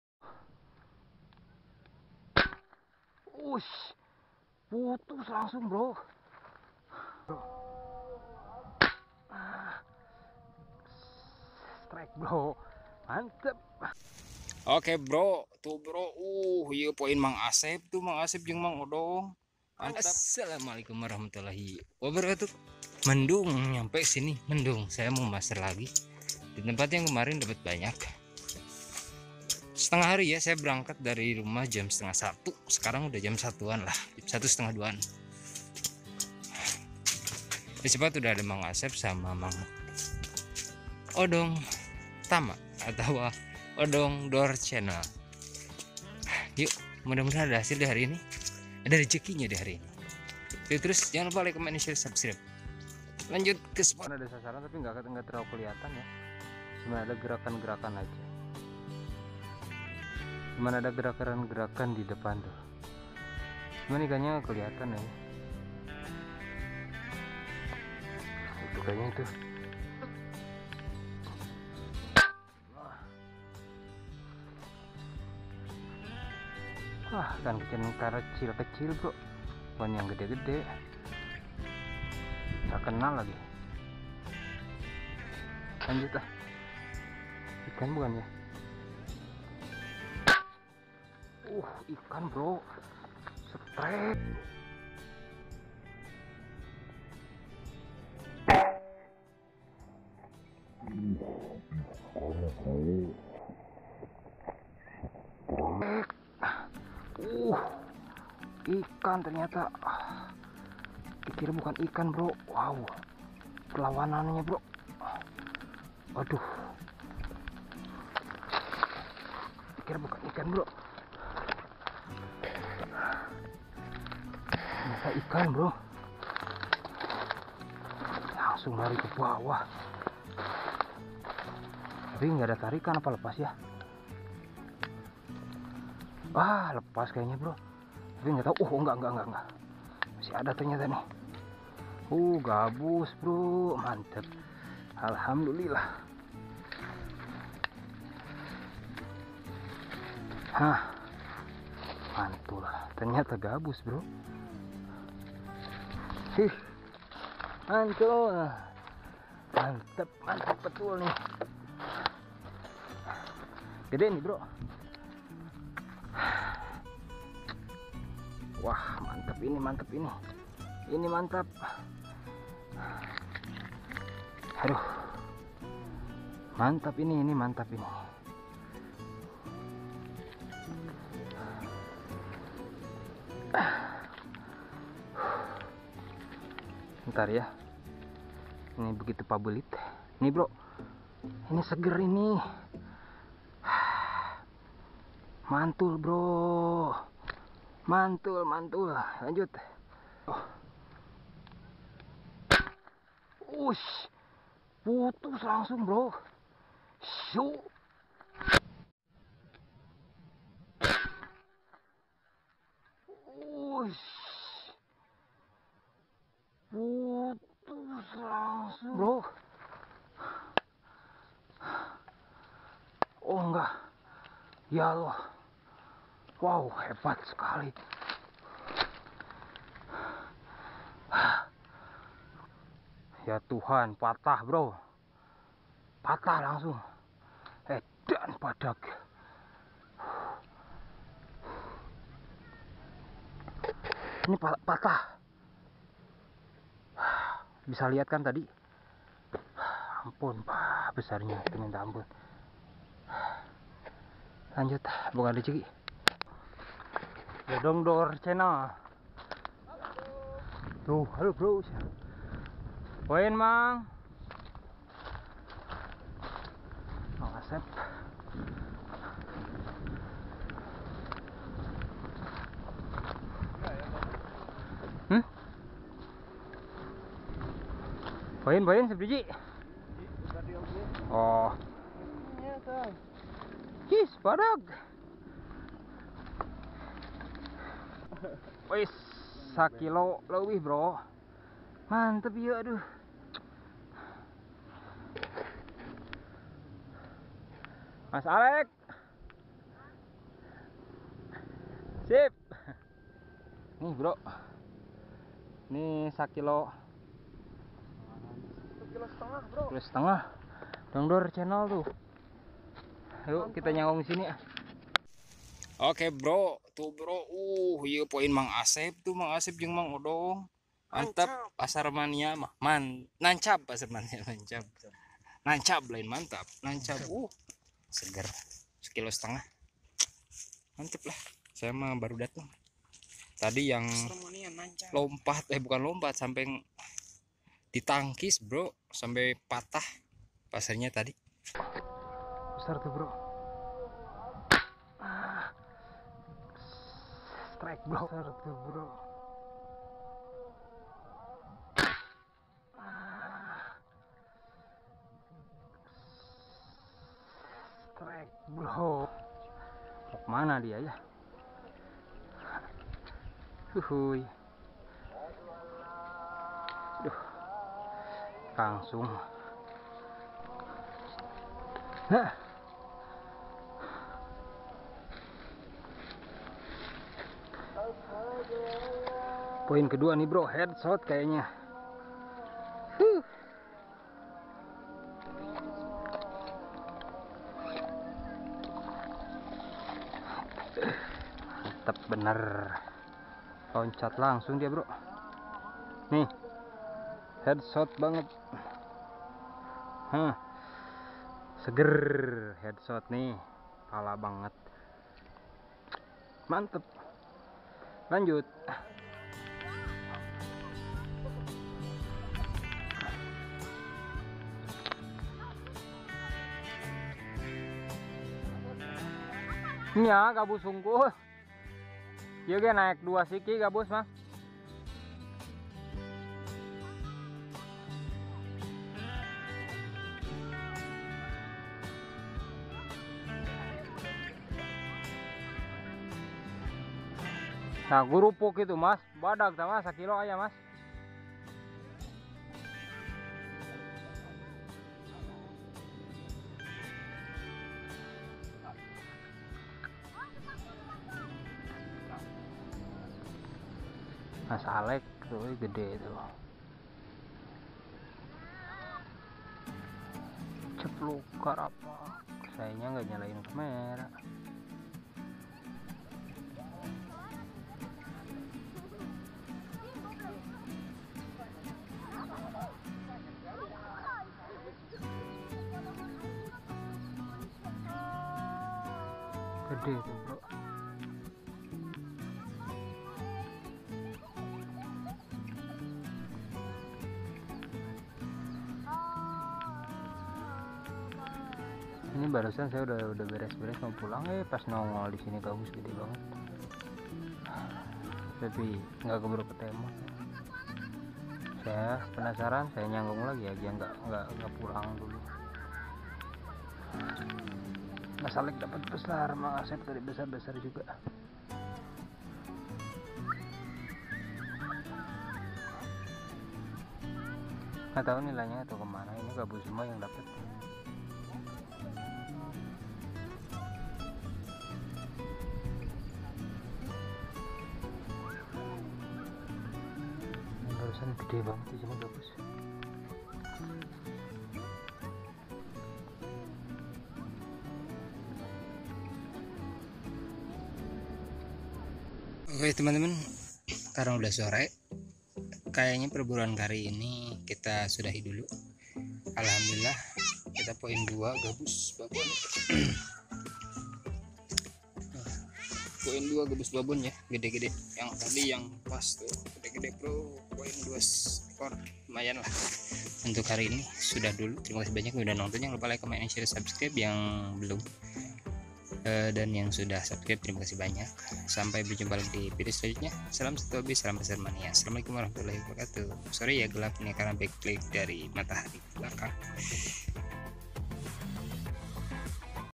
TA DI SANGKA MATA PASER SAMPAI PATAH#fishunting
kita maser lagih di spot yg penuh eceng gondok dan ta di sangka mata paser sampe patah saat nge shot ikan besar
salam paser mania